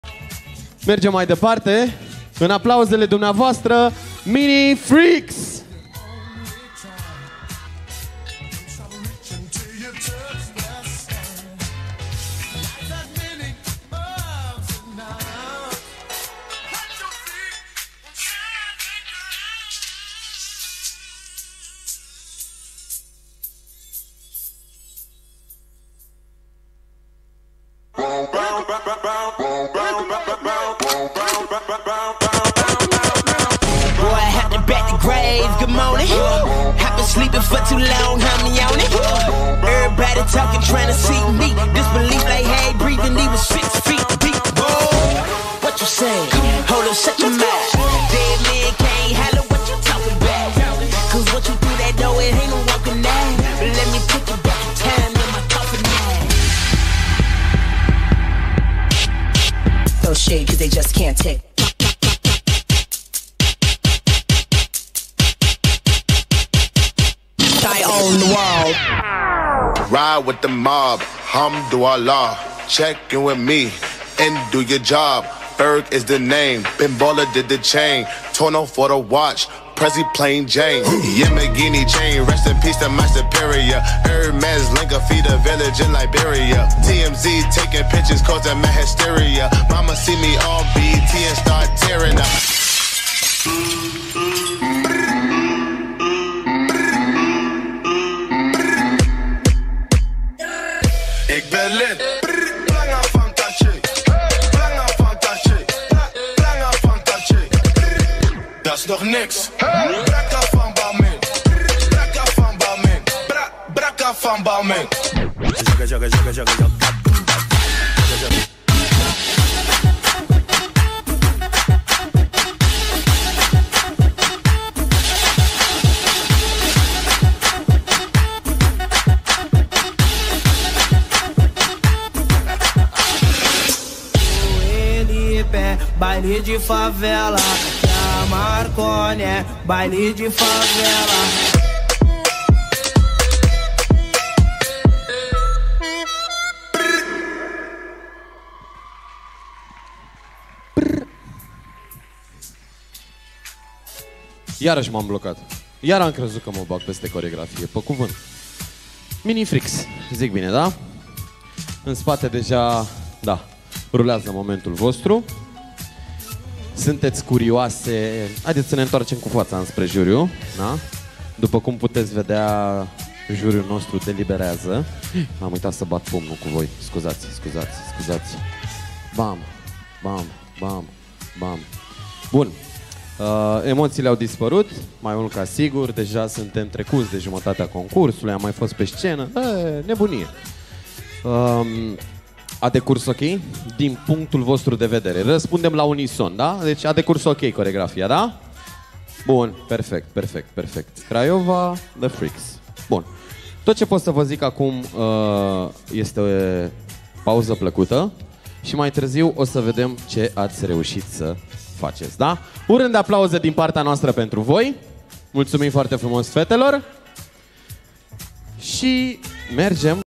Mini Freaks - 1st place (Small Team) | Campionatul National de Hip Hop/Street Dance 2021 FRDS
Campionatul National de Hip Hop / Street Dance
07 mai 2021 - Timisoara

Organizator: Federatia Romana de Dans Sportiv

Trupa: Mini Freaks - Freaks Dance Company
Disciplina: Street Dance Show
Categoria: Small Team
Categoria de varsta: 10 - 18 ani
Premiu: 1st place